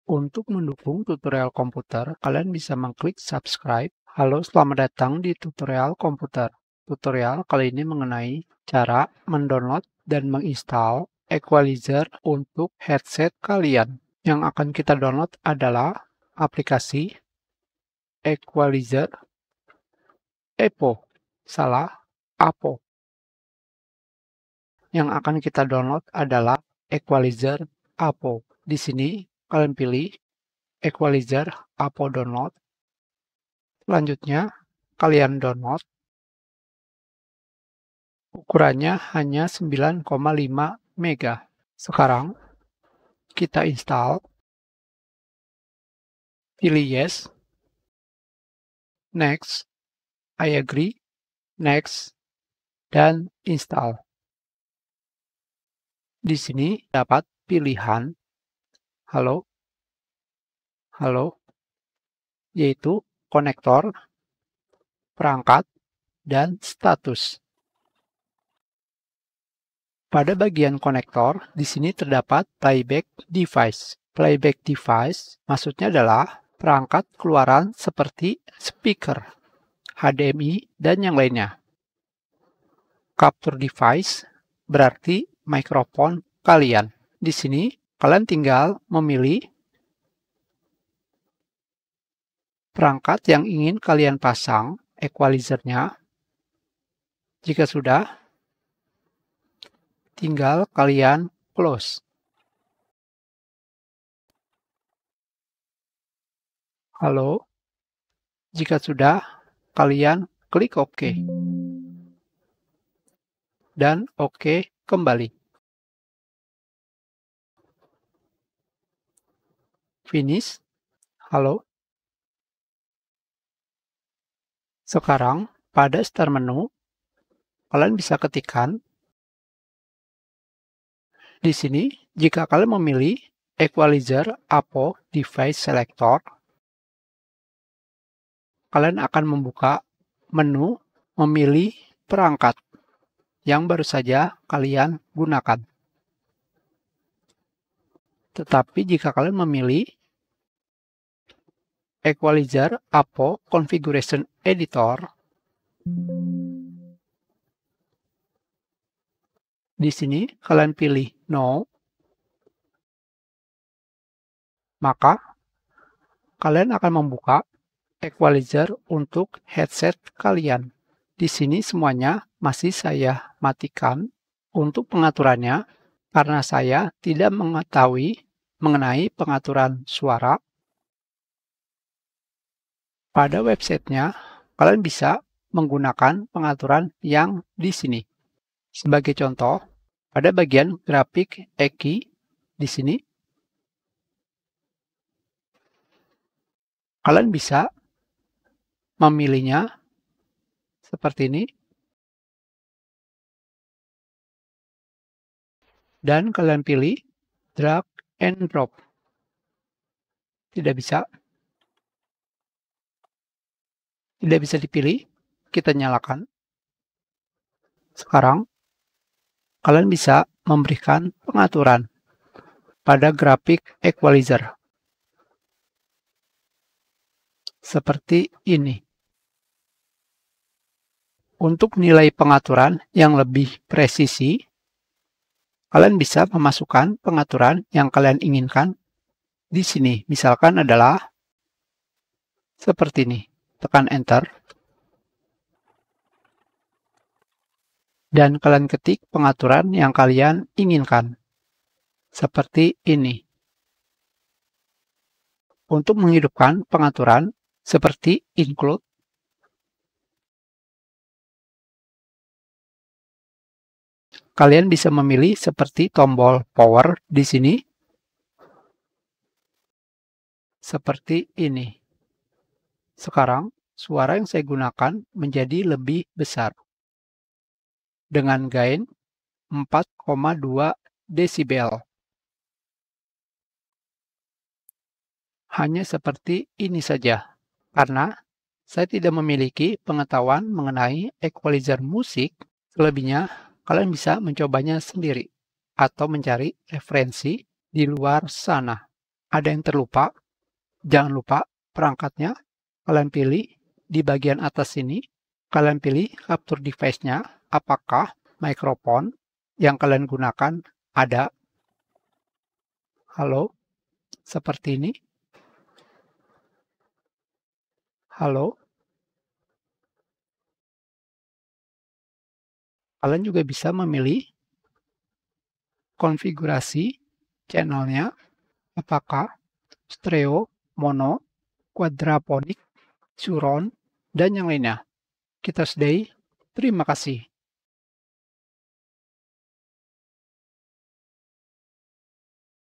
Tutorial Cara Download, Install, Pengaturan Equalizer APO, Equalizer Mic/Microphone Headset Gratis
Tutorial komputer kali ini mengenai cara download, install, pengaturan equalizer APO untuk pengaturan Microphone.

Bagian Video:
00:00 Cara Download Equalizer APO
01:00 Cara Pengaturan Konekstor dan Playback Device
02:03 Cara Mengatur Equalizer Microphone